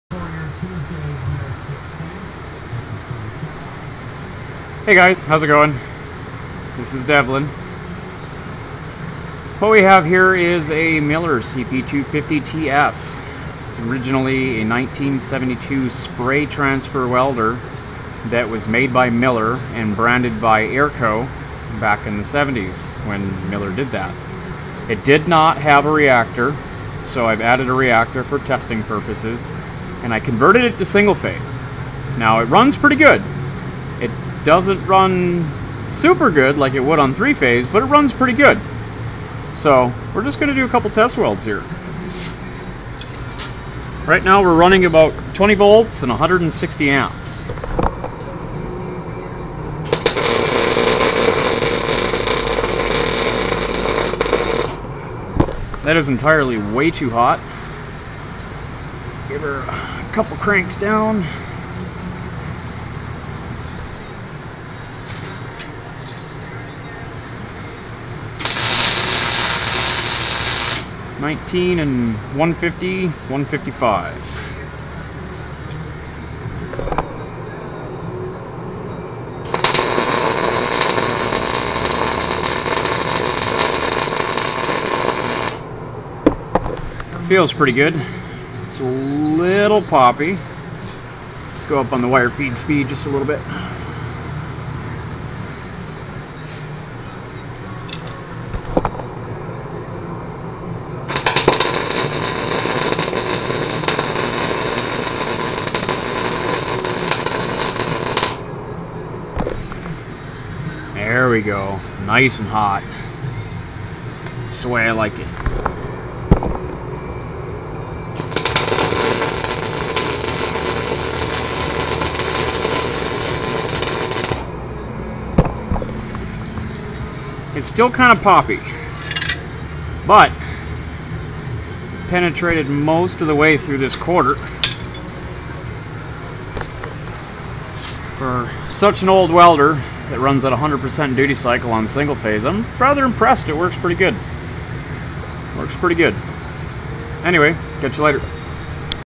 Miller CP 250 TS running single phase 240v and short arc (Airco 2.5DTR-224-A)
I got this power supply and feeder for a good price, converted it to single phase, cleaned and fixed various parts and added a reactor (inductor) coil out of a CP 302. Next is a capacitor bank and a few other odds and ends, maybe digital readouts. I might make a video on how I did it.